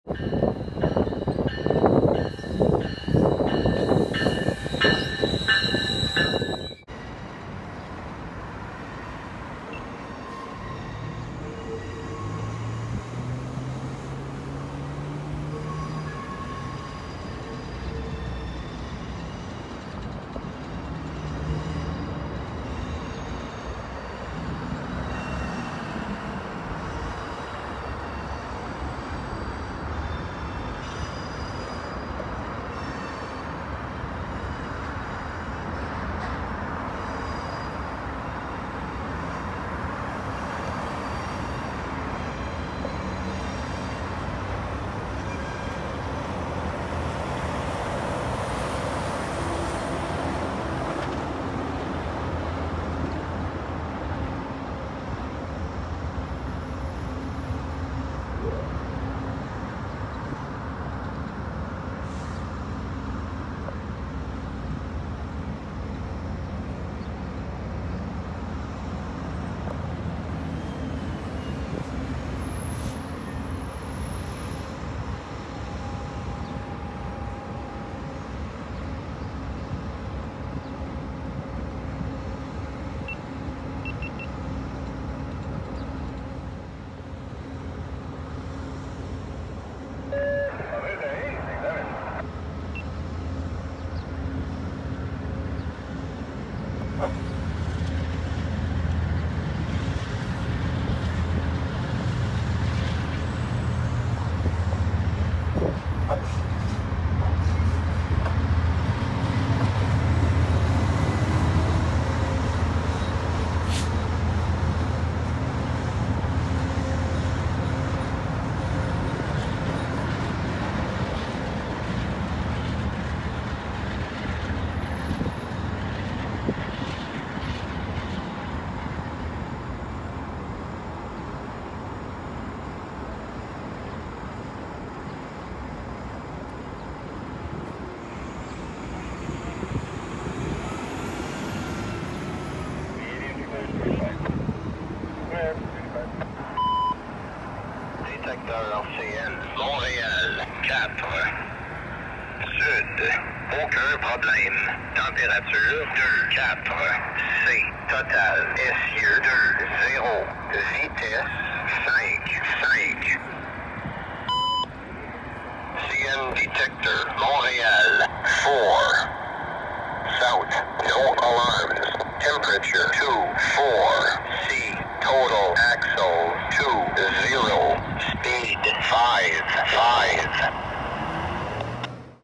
VIA 35 Reversing Out of Gare Centrale (JUL/28/2021)
VIA 35 from Quebec City to Ottawa Reverses Out of Gare Centrale Station with a consist of Budd Coaches and VIA 905.